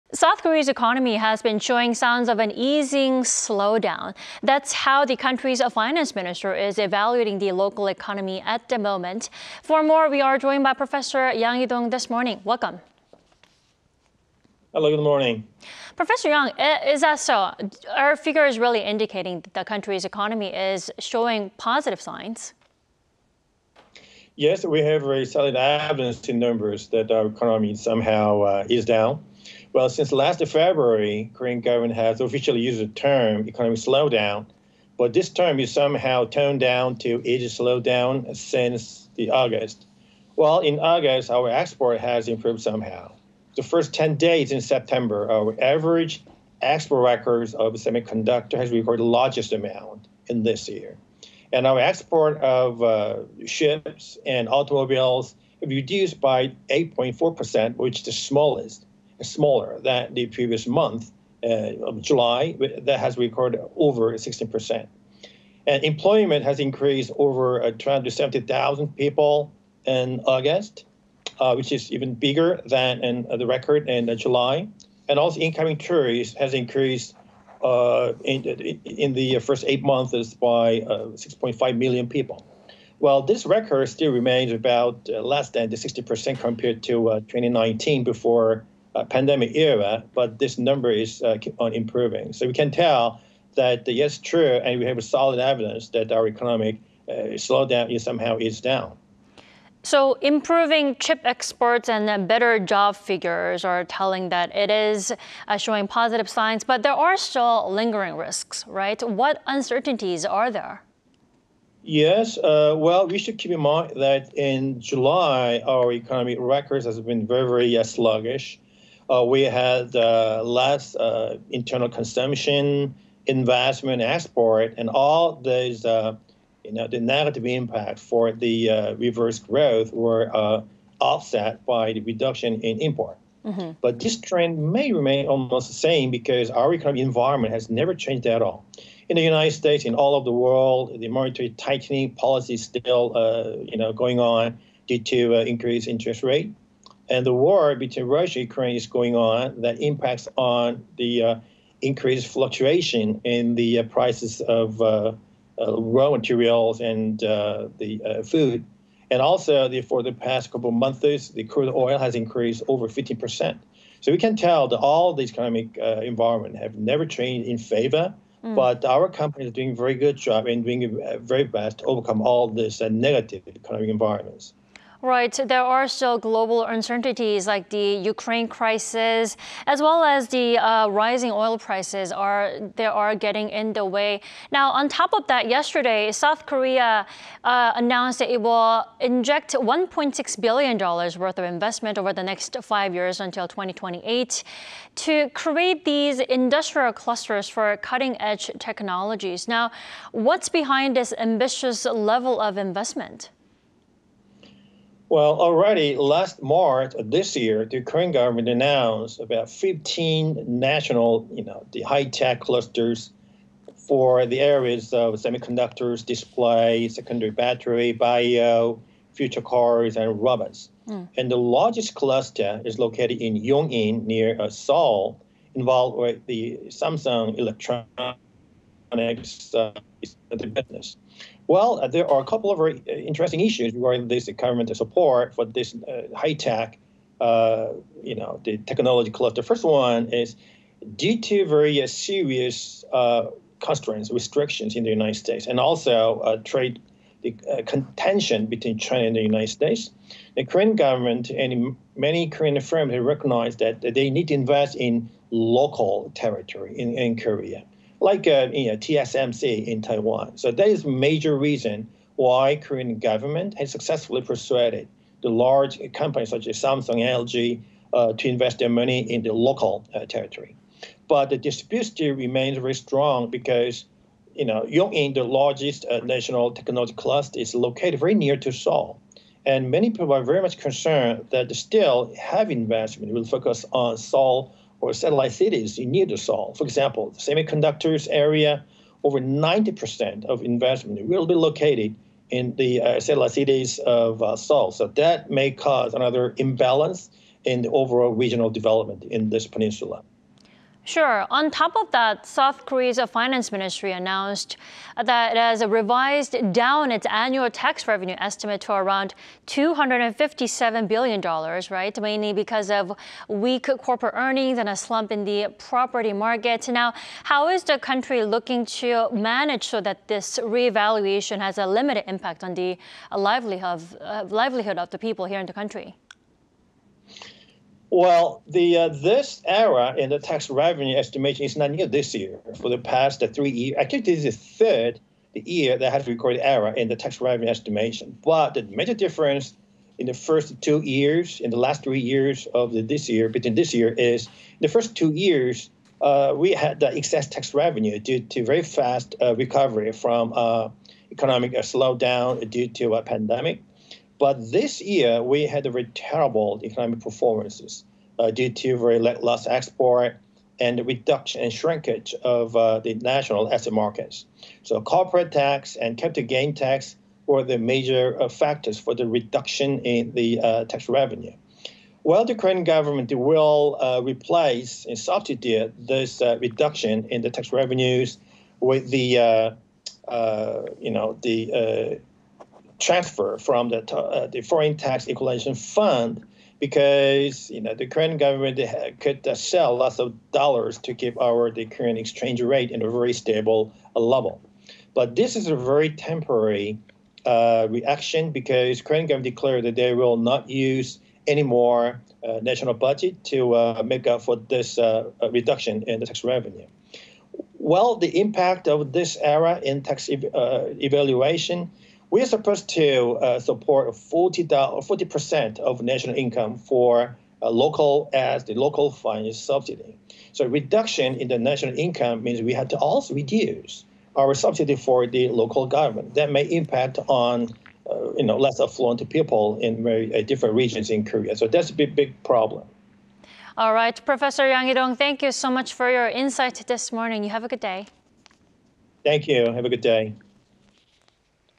On-point: S. Korean economy shows signs of eased slowdown on back of improving chip exports ...
On-point: S. Korean economy shows signs of eased slowdown on back of improving chip exports, better job data

추경호 "경기둔화 완화" 첨단클러스터 2.2조 투자

South Korea's economy has been showing signs of an easing slowdown.
That's how the country's finance minister is evaluating the local economy at the moment.
For more, we're joined by Professor Yang Hee-dong this morning.

Welcome.

1. Is that so? Do figures indicate that the country's economy is showing positive signs?

2. What uncertainties still remain though?

3. South Korea is planning 1 point 6 billion dollars worth of investment over the next five years to create industrial clusters for cutting-edge technologies.
What's behind this ambitious level of investment?

4. South Korea's finance ministry announced yesterday that it has revised down its annual tax revenue estimate to around 257 billion dollars mainly because of weak corporate earnings and a slump in the property market.
Is this a concerning issue?

5. Then how is the country looking to manage so that the reevaluation has a limited impact on the livelihood of the people?

Thanks so much for your insight this morning.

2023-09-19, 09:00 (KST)